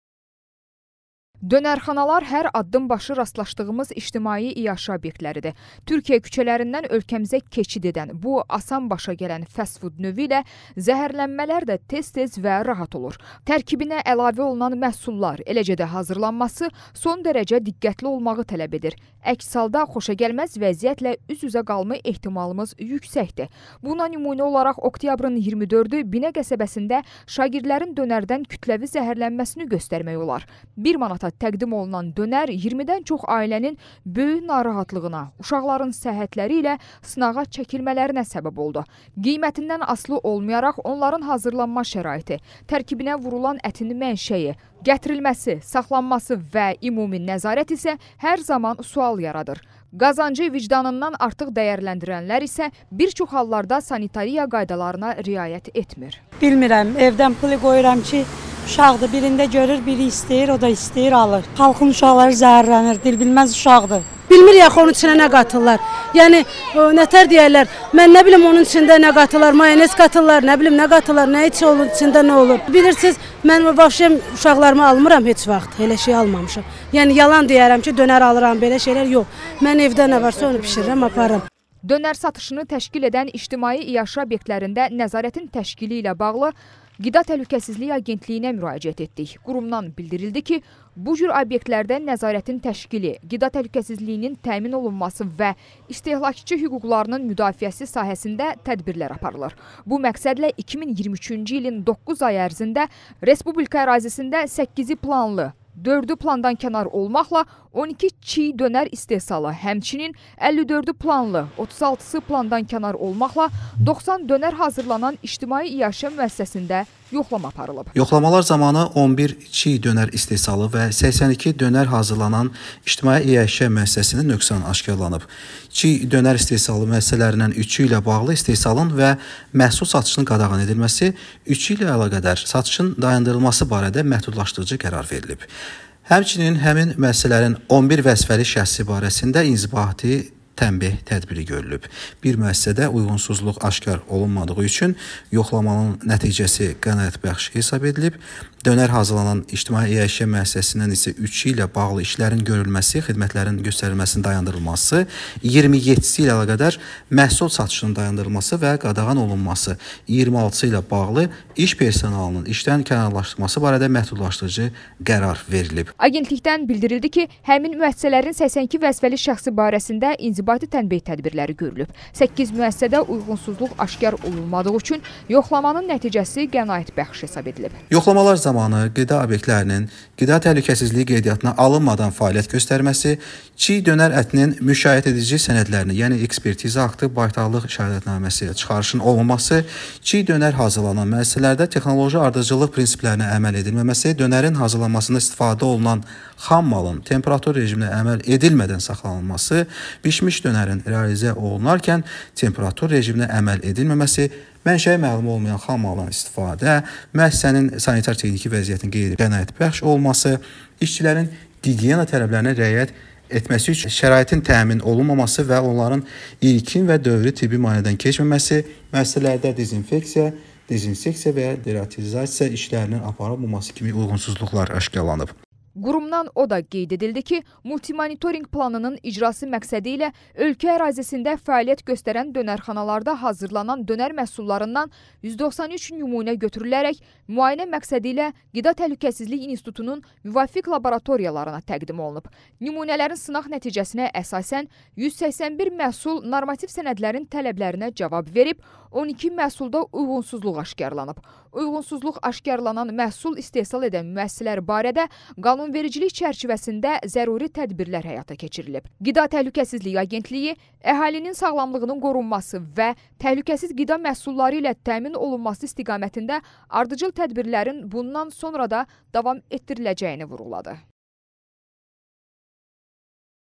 Dönərxanalarda qida təhlükəsizliyi
AQTA: 9 ay ərzində 11 vəzifəli şəxs barəsində inzibati tənbeh tədbiri görülüb